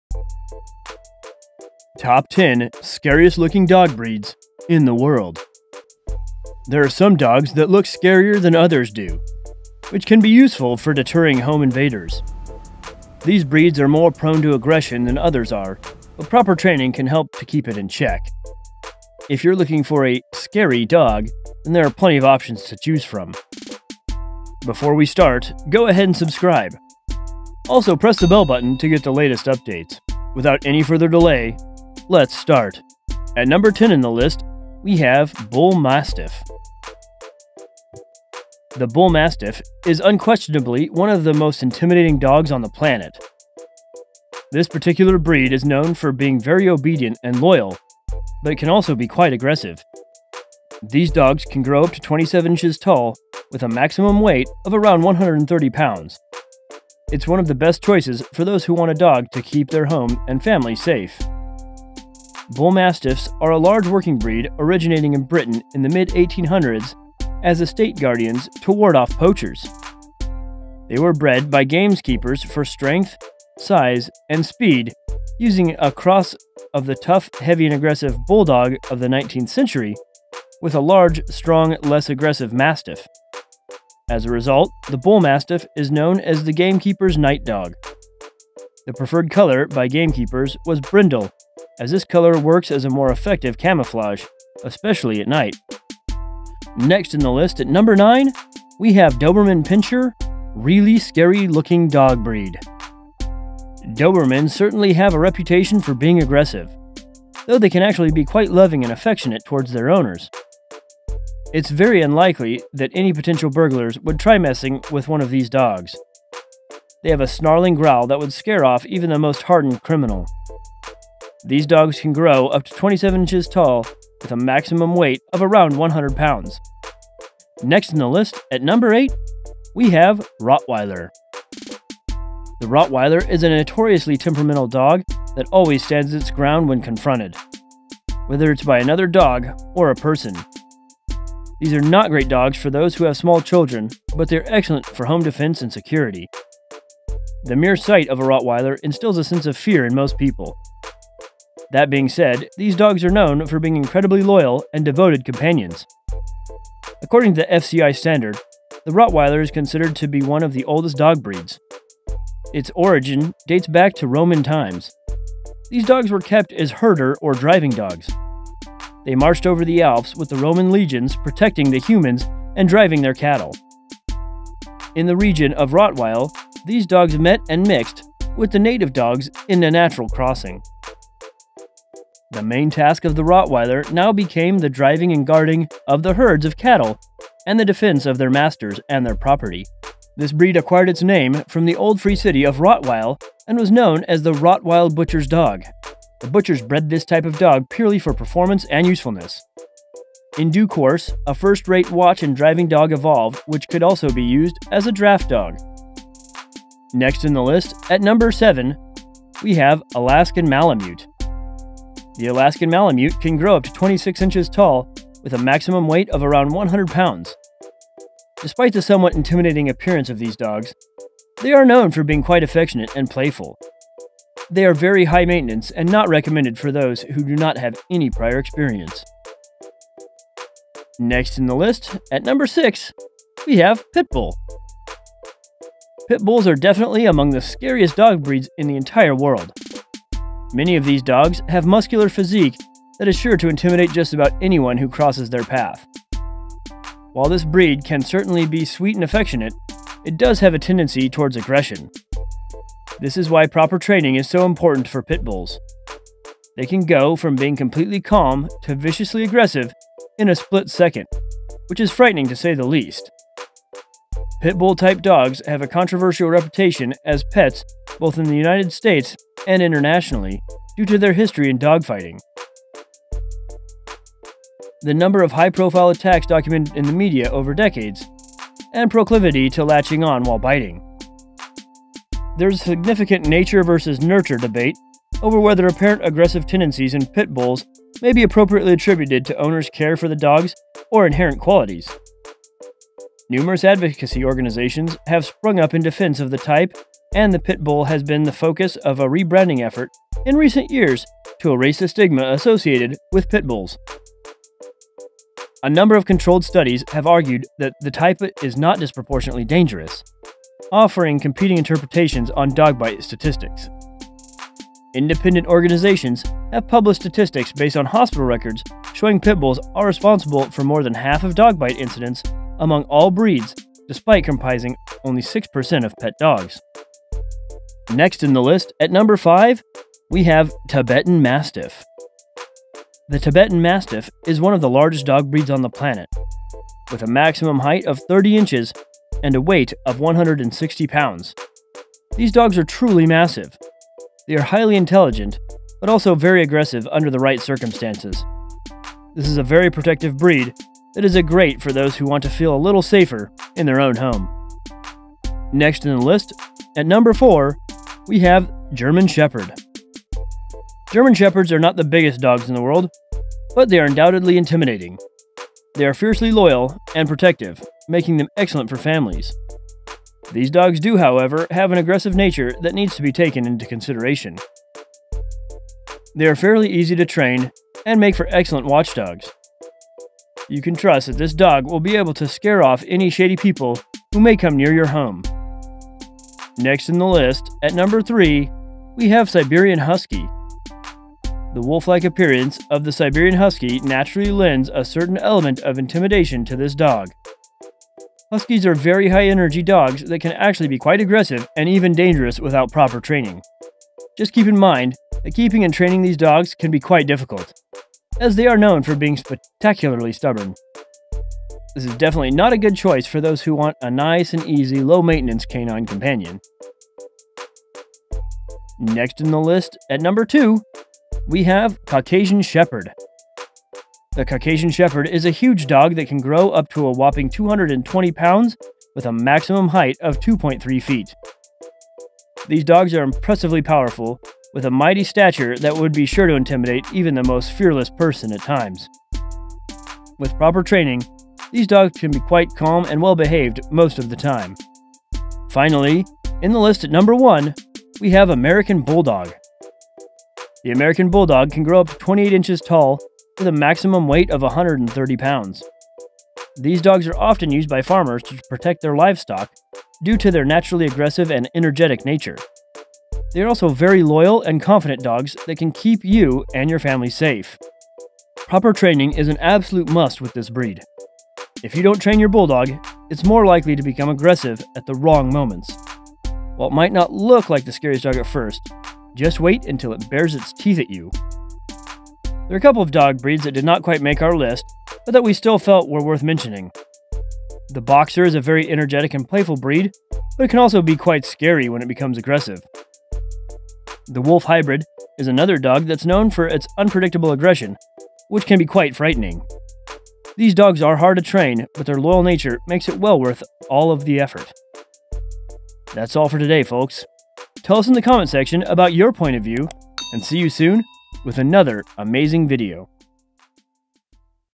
Top 10 Scary Looking Dog Breeds
Even though most dogs are as cute as your first born, some are looking like the devils' pet. These are some scary looking dogs